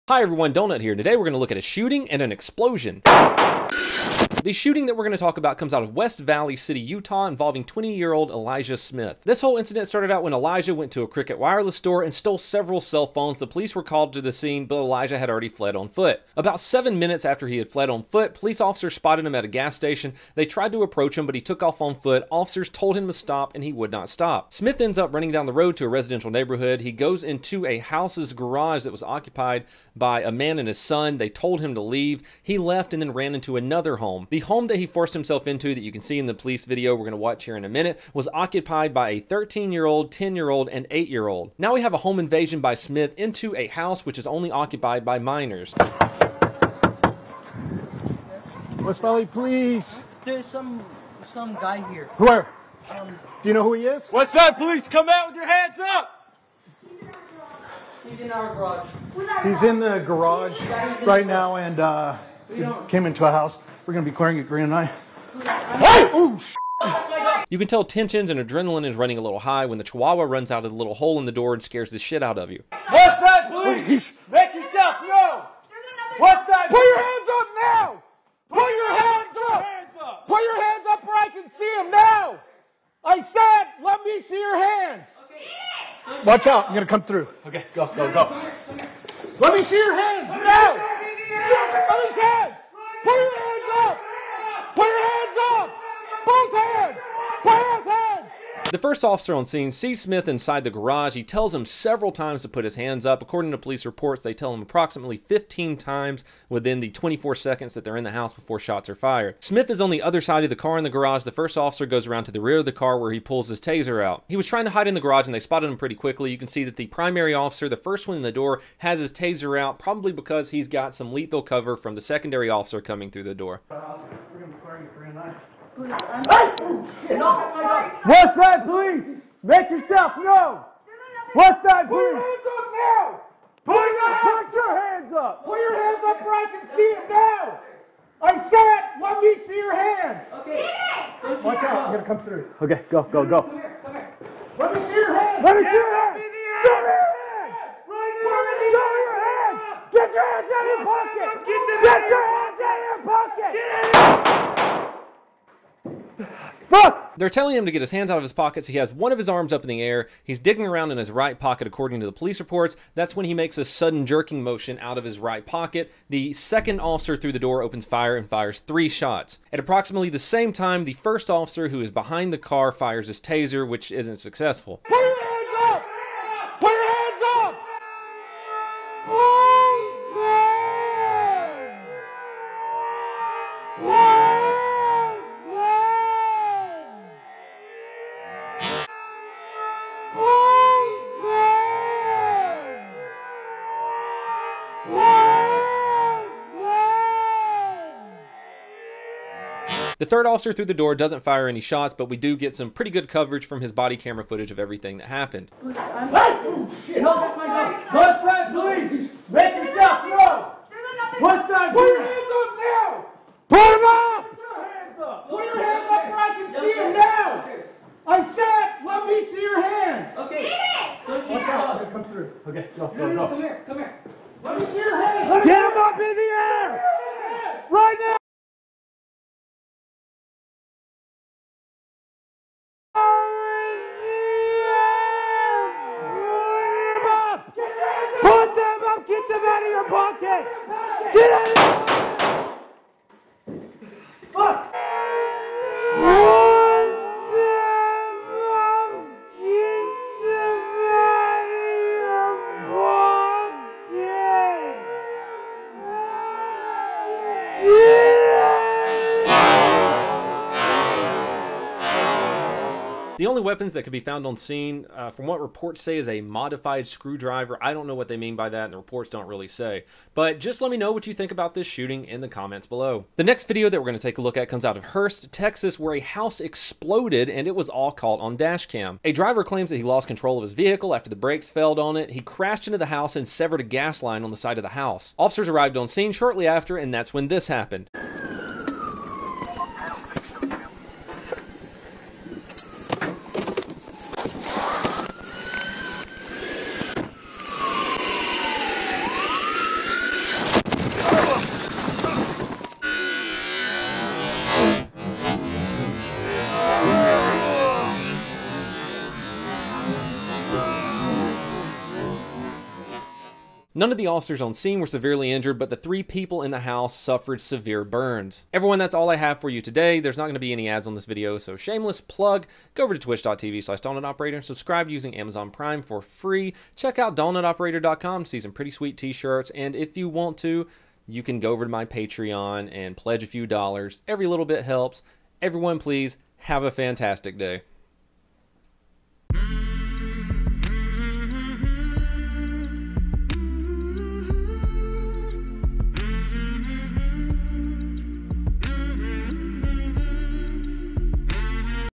Elijah Smith shooting in depth breakdown
Let's break down the shooting of 20-year-old Elijah Smith in West Valley City, Utah.

3300 N. Main St. D
PMB 282

*Gear*
Main Camera
Vlog Camera
PC:
Video card
Hard drive